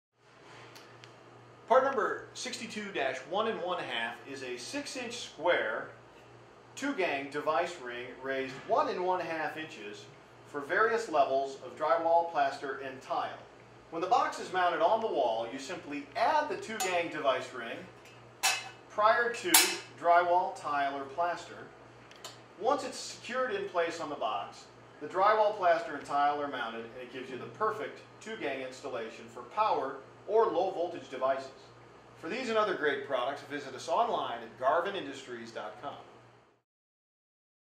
Square Box Cover: 1-1/2" Raised 6" Square One Gang Device Covers 62-1-1/2
- 1-1/2" raised device covers are used to mount numerous electronic and electrical devices to 6" square boxes. Garvin Industries' one gang device covers quickly and securely mount to boxes and provide access to install switches, devices, receptacles and other electronic/electrical appliances.